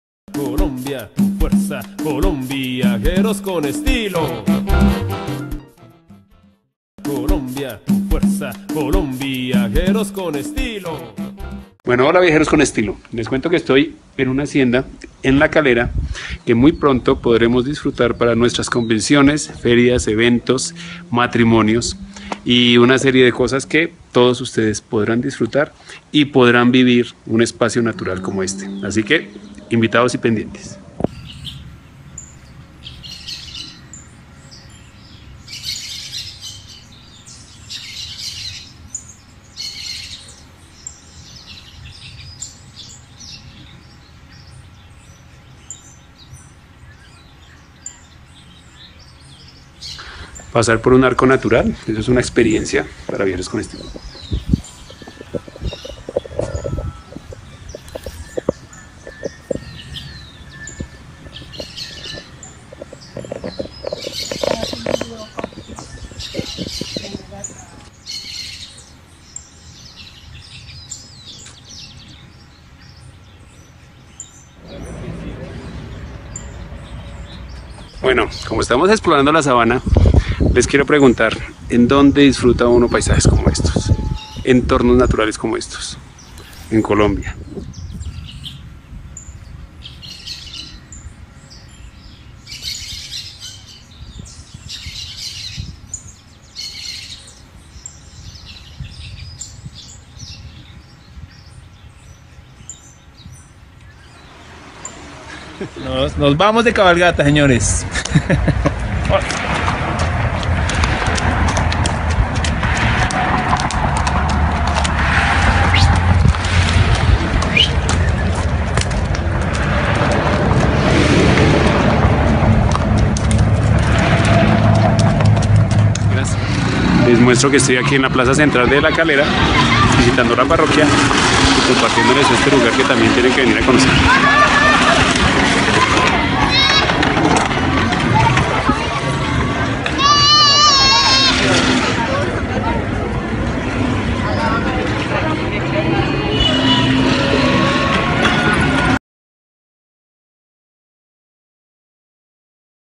RECORRIDO POR LA CALERA
David Legar nos muestra un poco de los atractivos que tiene La Calera, municipio de Cundinamarca muy cerca de Bogotá y el hermoso sitio dónde llevaremos a cabo diferentes actividades para Viajeros con Estilo!